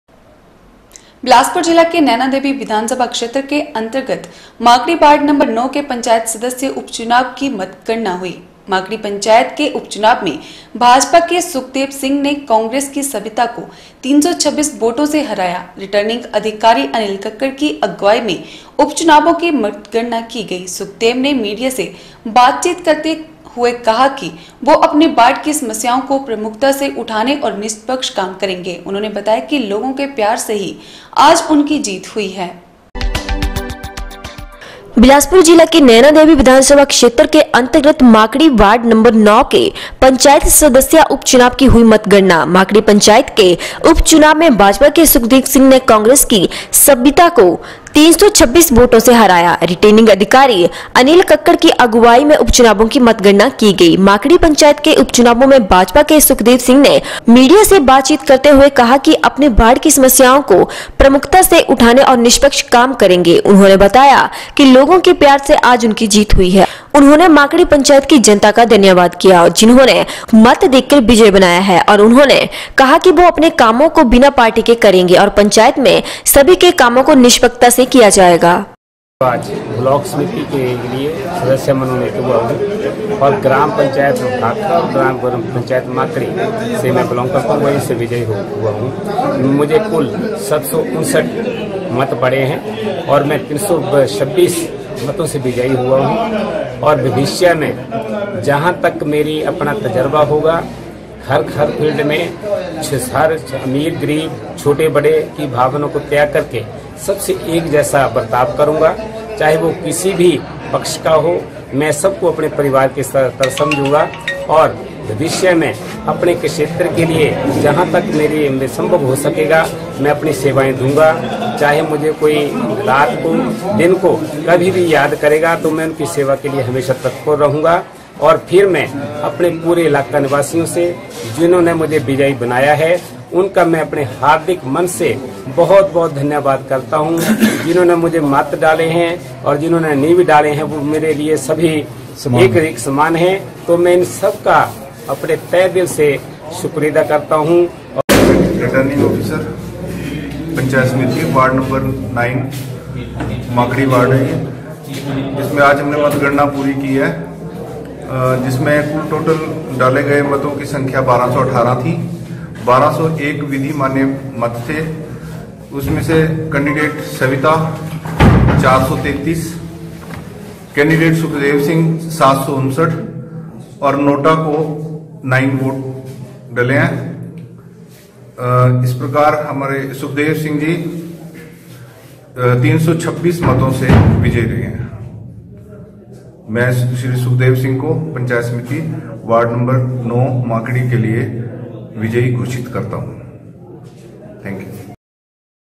19 NOV N 6 नैनादेवी वार्ड नम्बर 09 के पंचायत सदस्य उपचुनाव की हुई मतगणना
बिलासपुर जिला के नैनादेवी विधानसभा क्षेत्र के अंतर्गत माकड़ी वार्ड नम्बर 09 के पंचायत सदस्य उपचुनाव की हुई मतगणना, माकडी पंचायत के उपचुनाव में भाजपा के सुखदेव सिंह ने कांग्रेस की सविता को 326 वोटो से हराया रिटर्निंग अधिकारी अनिल ककड़ की अगवाई मै उपचुनावों की मतगणना की गईसुखदेव देव ने मीडिया से बातचीत करते हुए कहा कि वो  अपने वार्ड की समस्याओं को प्रमुखता से उठाने और निष्पक्ष काम करेंगे उन्होंने बताया कि लोगो के प्यार से आज उनकी जीत हुए